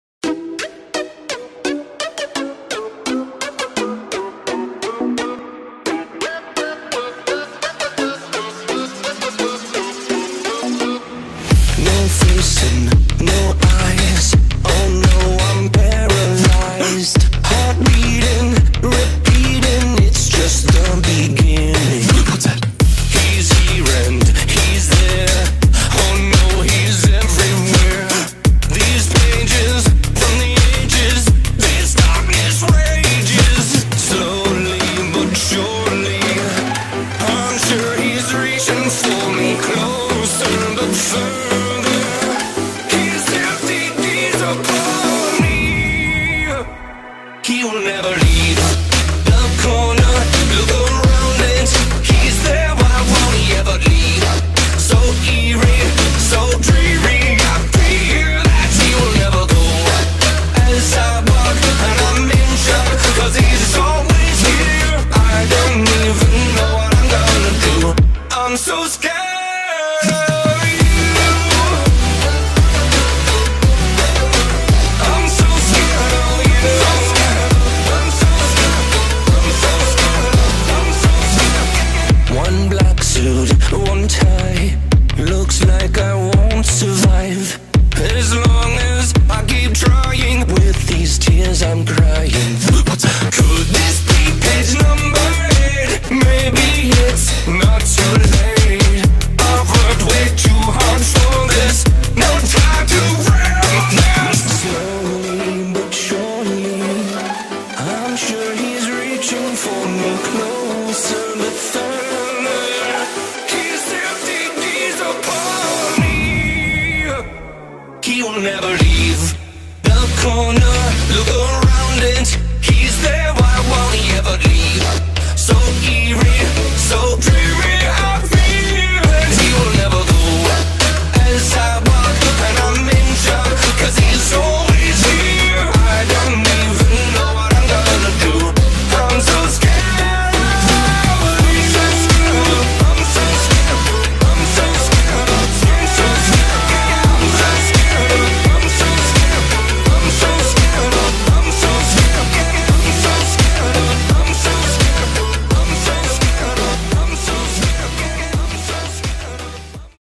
Trying a Slenderman Artwork (Sorry if it's ugly) || Art w/ IbisPaint
Apps used in this video:
Ibis Paint
Capcut